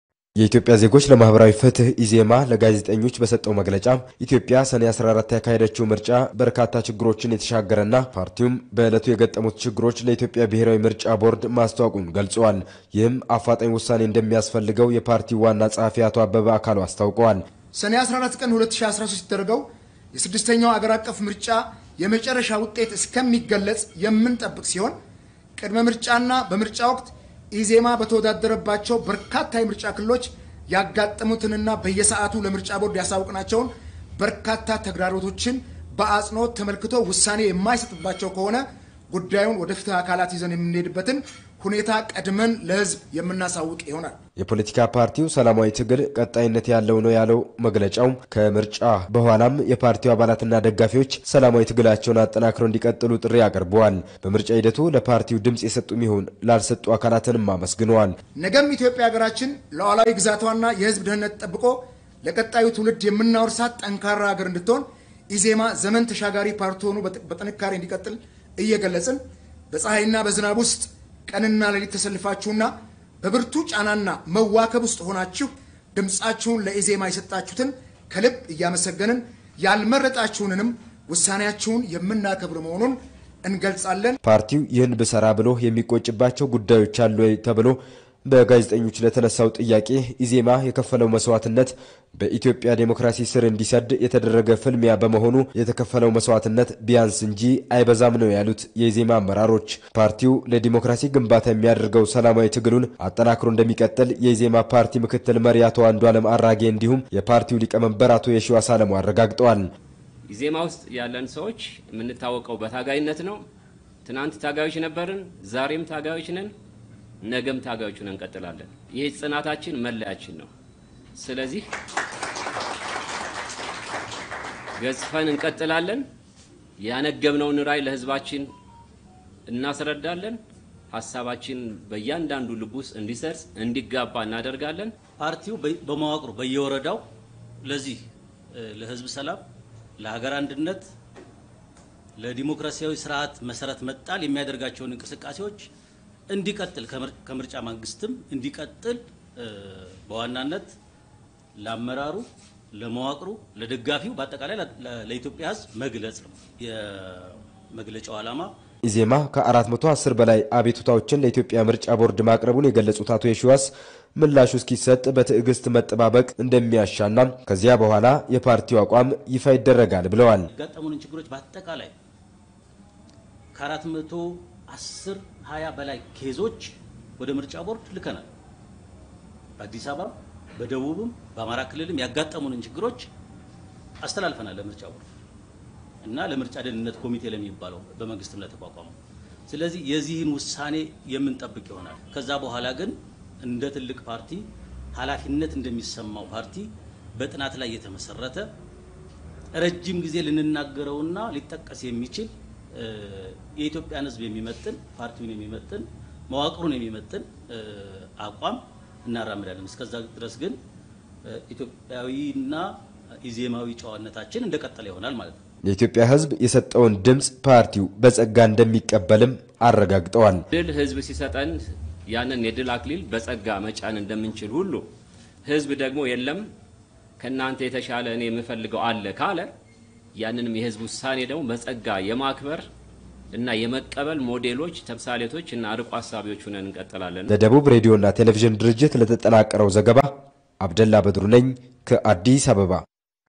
የኢትዮጵያ ዜጎች ለማህበራዊ ፍትህ /ኢ.ዜ.ማ/ በሰላማዊ እና በህጋዊ መንገድ ብቻ የፖለቲካ ትግሉ እንደሚቀጥል አስታወቀ
ዘጋቢ፡ አብደላ በድሩ